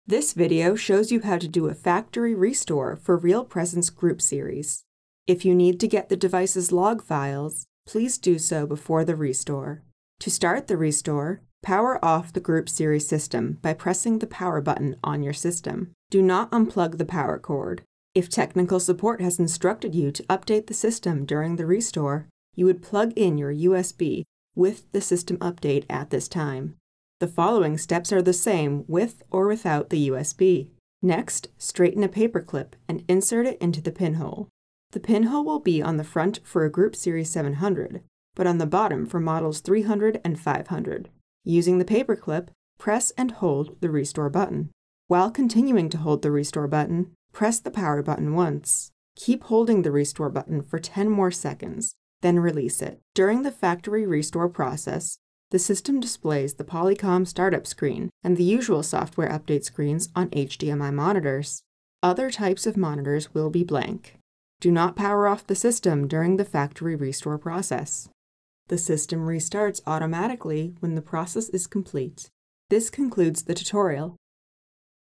How to Factory Restore a Group Series (with or without USB) | HP Support
This video shows you how to do a Factory Reset for Group Series.